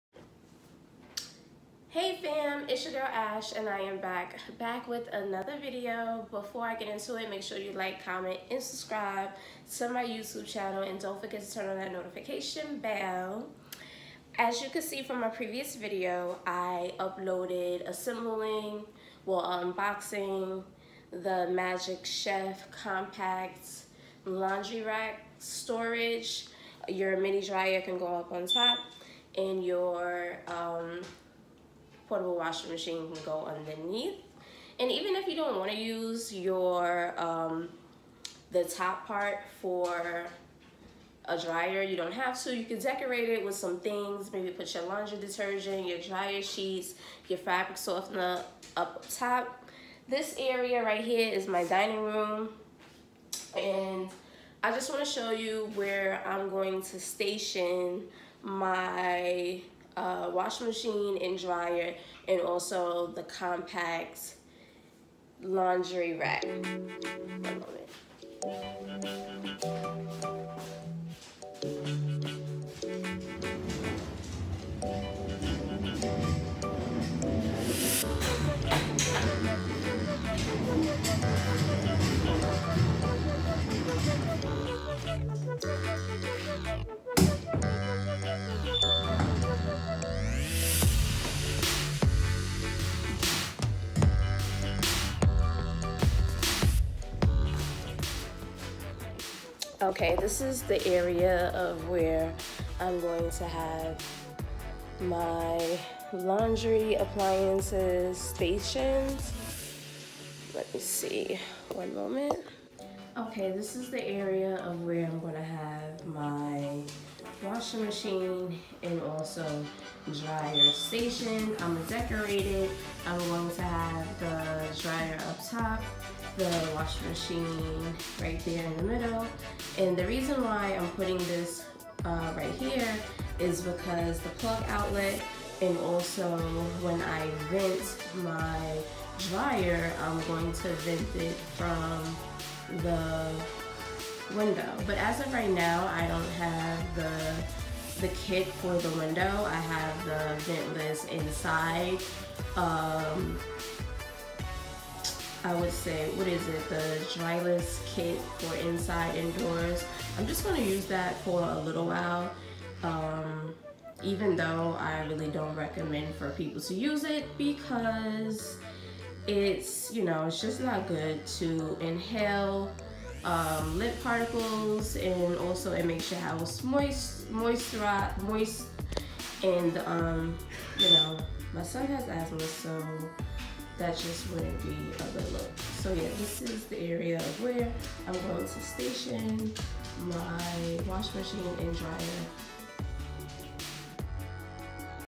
HOW TO: Create space for my Portable Dryer & Washing Machine 🤷🏽
Hey Fam, so now that I have my portable dryer & washing machine, now it’s time to create space by using “The Magic Chef Compact Laundry Rack”.